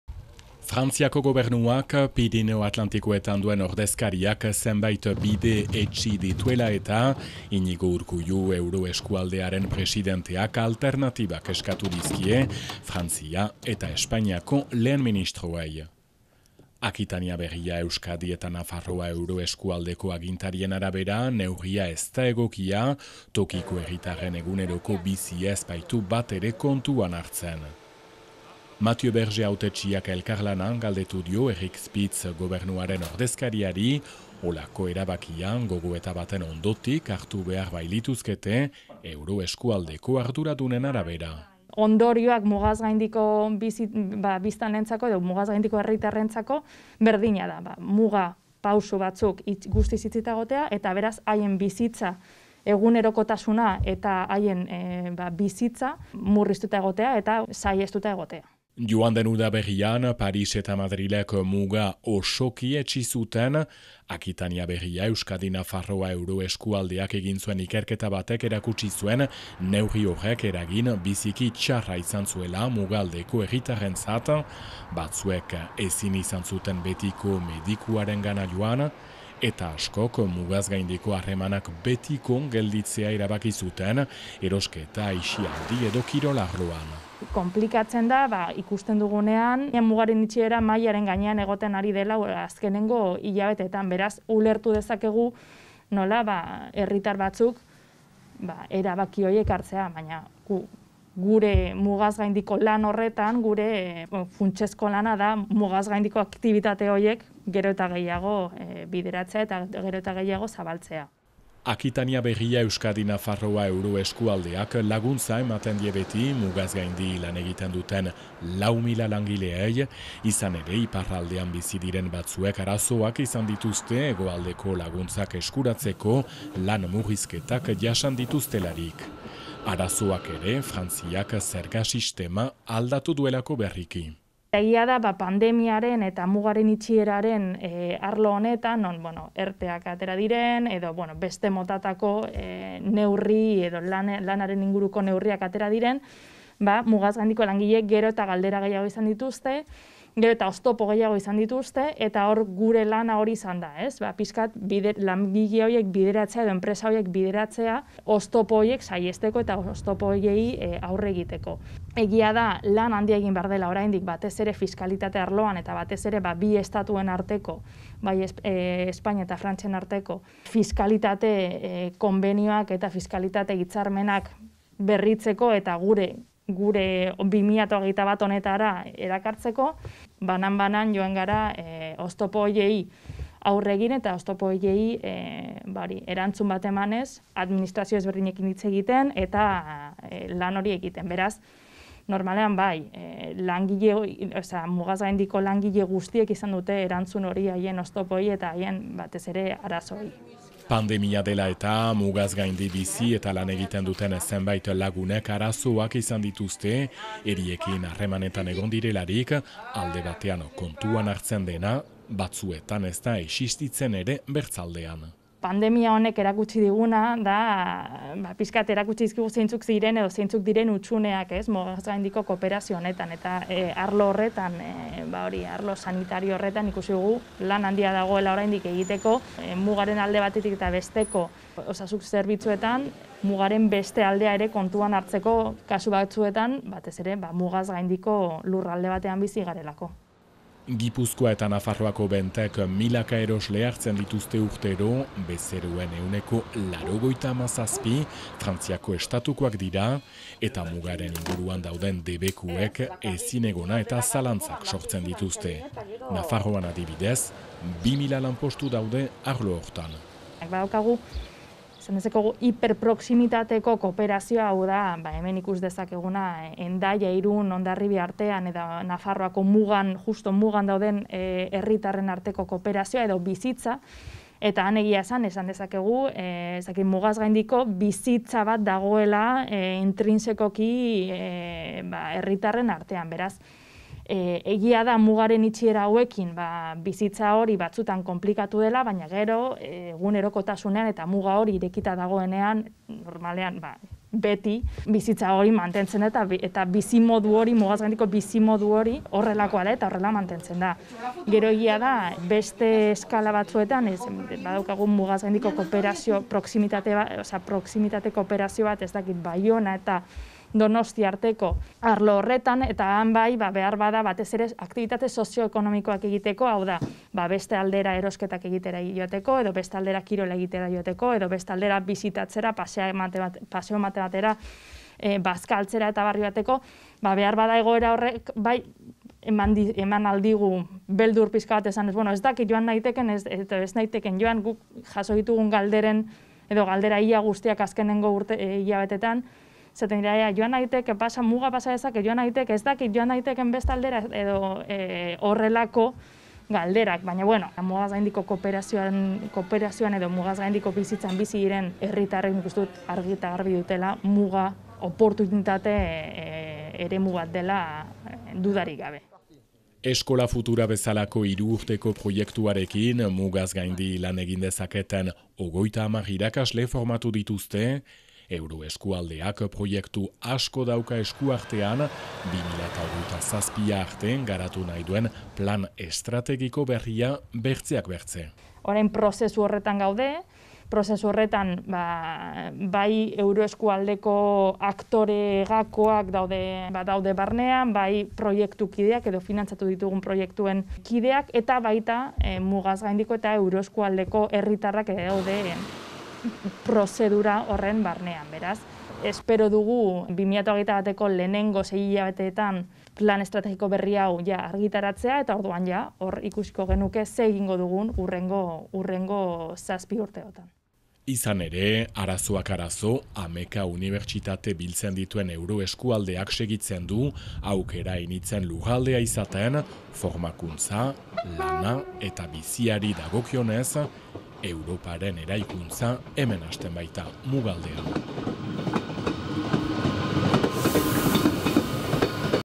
Arola Urdangarin
Solasean Akitania Berria/Euskadi/Nafarroa euroeskualdearen zuzendariarekin.